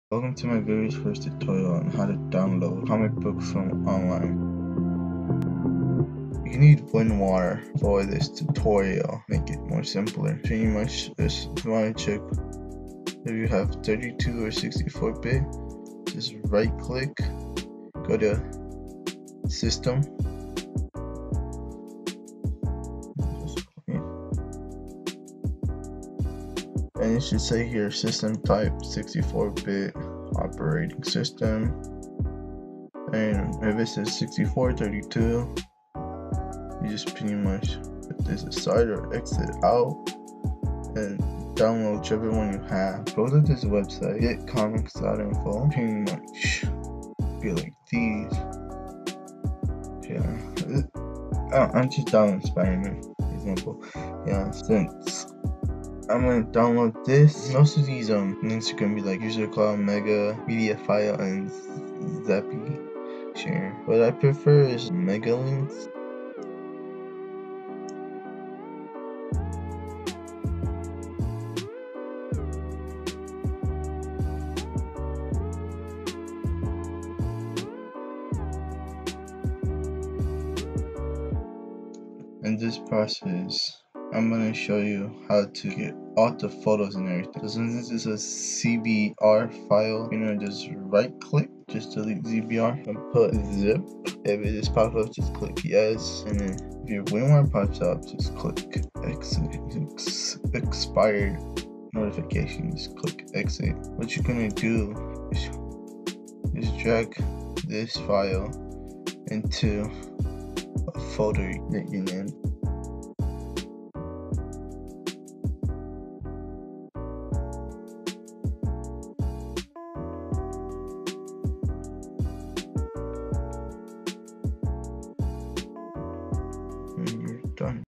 How to Download Comics Online (NO CONVERTER NEEDED)
This my very first tutorial, so that explain why I'm kinda nervous or what not. I never got to finished editing this til today which it explain why it was record in March.